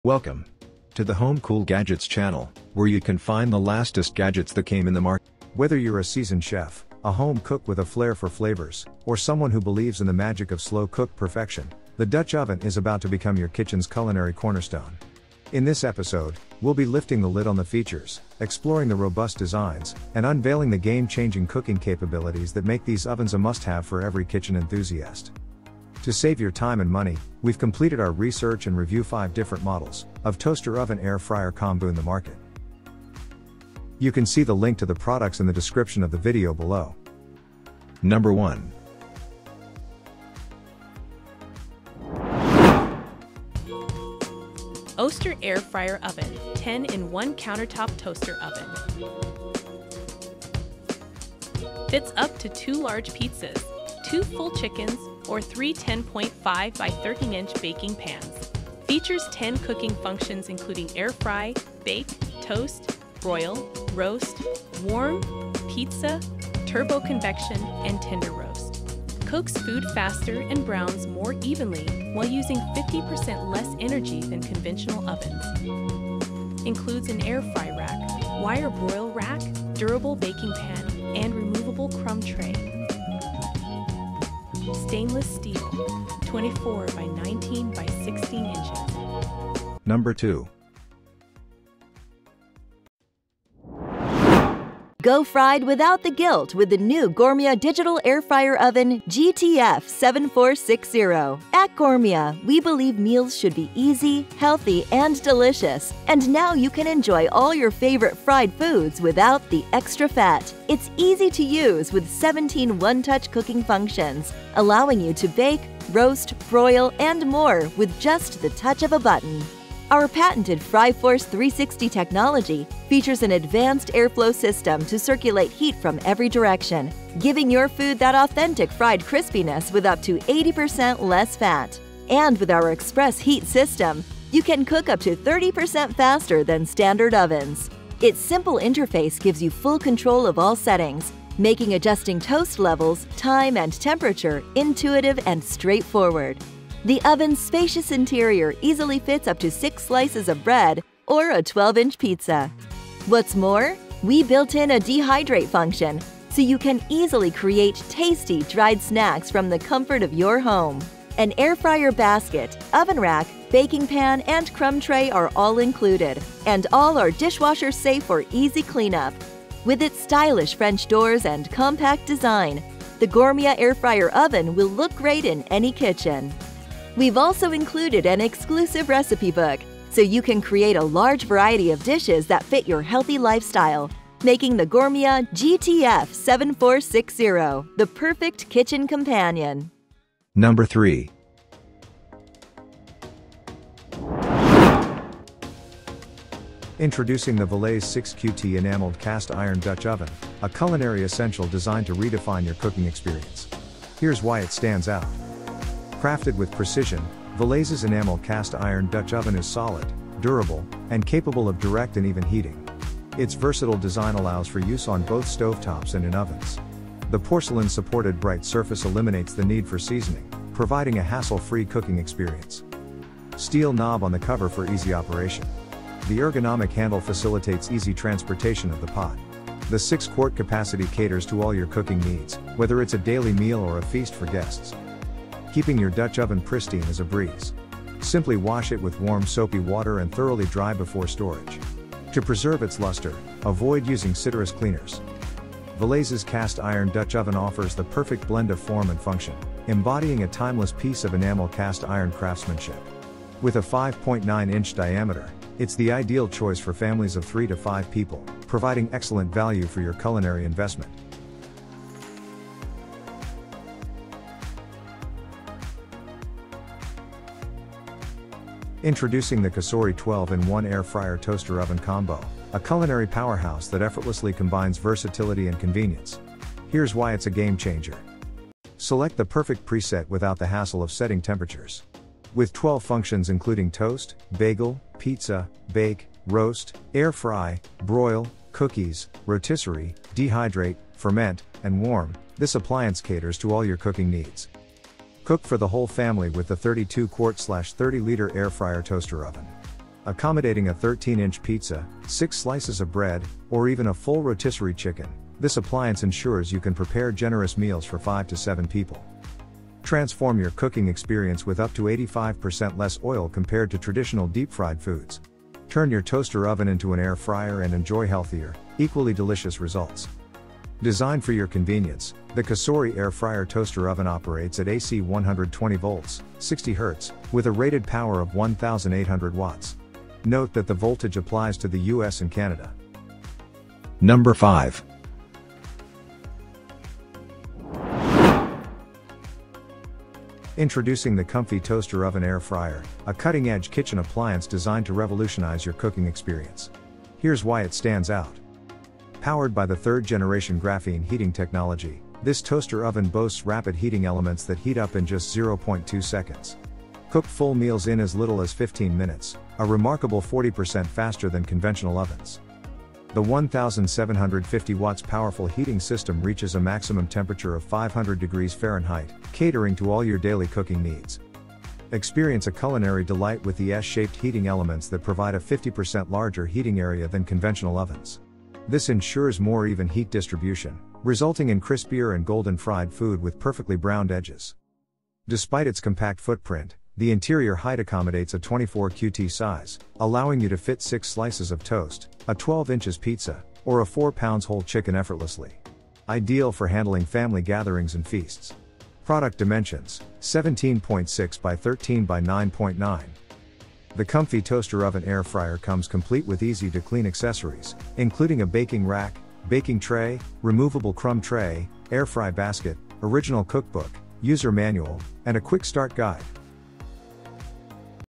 Top 5 Best Toaster Oven Air Fryer Combo Amazon 2024 Countertop Toaster
Top 5 Best Toaster Oven Air Fryer Combo Amazon 2024 Countertop Toaster Oven

Here Is The Link For Toaster Oven Air Fryer Combo 👇👇

Oster Countertop Toaster Oven Air Fryer Combo
Gourmia Toaster Oven Air Fryer Combo
KitchenAid Digital Countertop Oven with Air Fry
COSORI 12-in-1 Air Fryer Toaster Oven Combo
COMFEE' Toaster Oven Air Fryer Combo

😍 Offers & Deals 😍
Amazon Prime For 2 Day Delivery
Create Amazon Business Account
Audible Gift Memberships
Prime Student 6-month Trial

00:00 Introduction
00:41 Oster Countertop Toaster Oven Air Fryer Combo
01:31 Gourmia Toaster Oven Air Fryer Combo
03:12 KitchenAid Digital Countertop Oven with Air Fry
04:26 COSORI 12-in-1 Air Fryer Toaster Oven Combo
05:37 COMFEE' Toaster Oven Air Fryer Combo